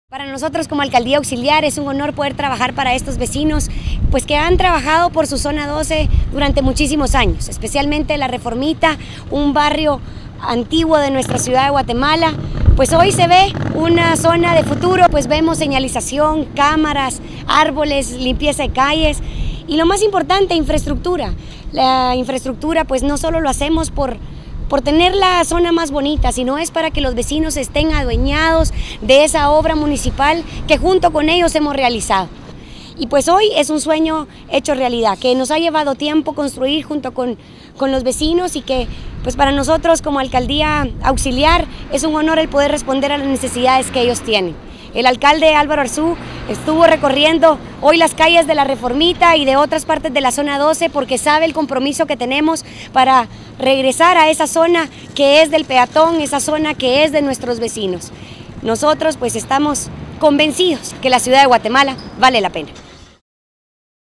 Renovamos La Reformita
Derivado del acercamiento de la Alcaldía Auxiliar de la Zona 12 con los vecinos, hemos desarrollado los trabajos necesarios para mejorar la infraestructura y calidad de vida en La Reformita.